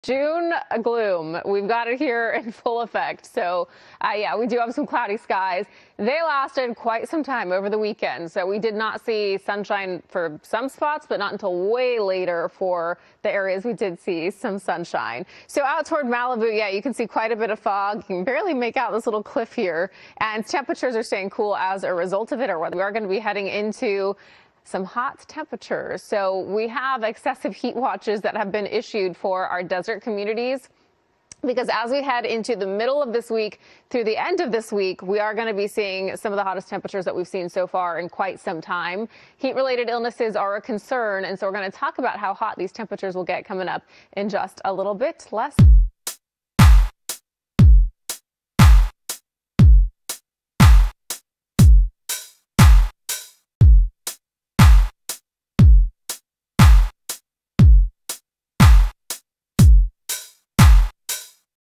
Brianna Ruffalo 1080p HD ❤️‍🔥 (viral No. 3)
Meteorologist, Brianna Ruffal provides the latest forecast. Bonus content at the end of the video.